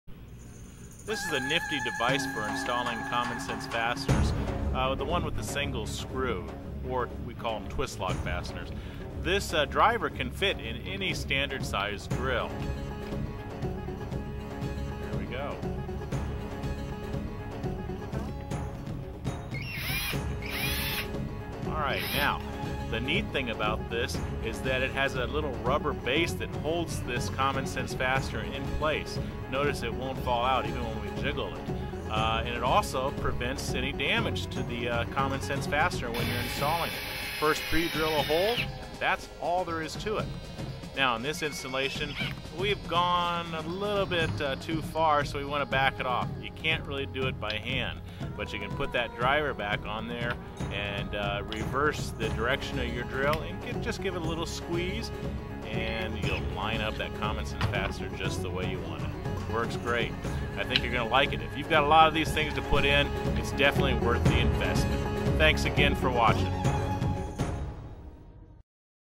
Common Sense Stud Single Screw Driver for Drill
Fits in your drill like any bit to quickly drive in Common Sense (Twist Lock) single screw studs.